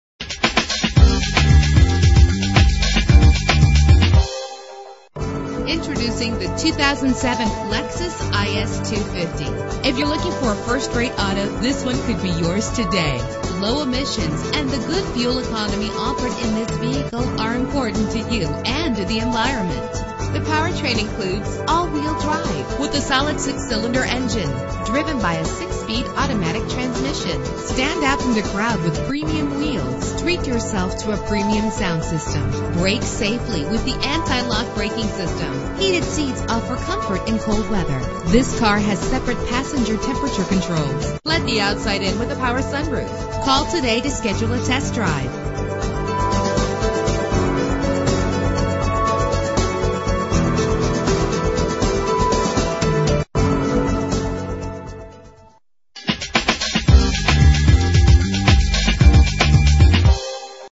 2007 Lexus IS 250 - Bedford OH
Year: 2007
Make: Lexus
Model: IS 250
Trim: Base
Engine: 2.5 liter 6 cylinder 24 valve
Transmission: 6-Speed Automatic
Color: Gray
Mileage: 71567
Address:
250 Broadway